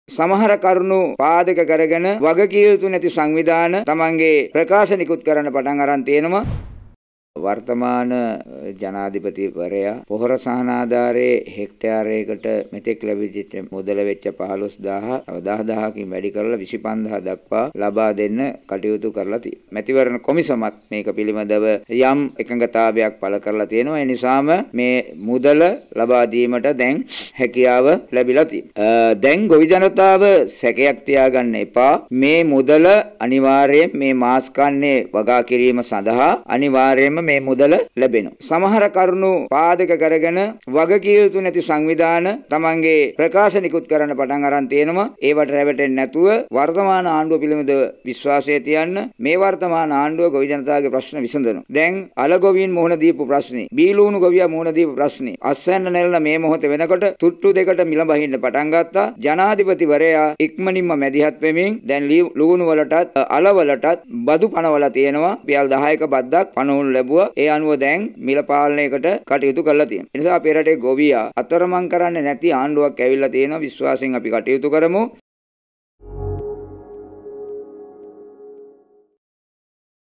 මැතිවරණ කොමිසම වැළැක්වූ පොහොර සහනාධාරය ජනතාවට හම්බවෙයිද? | Election Commission of Sri Lanka
මැතිවරණ කොමිසම වැළැක්වූ පොහොර සහනාධාරය ජනතාවට හම්බවෙයිද?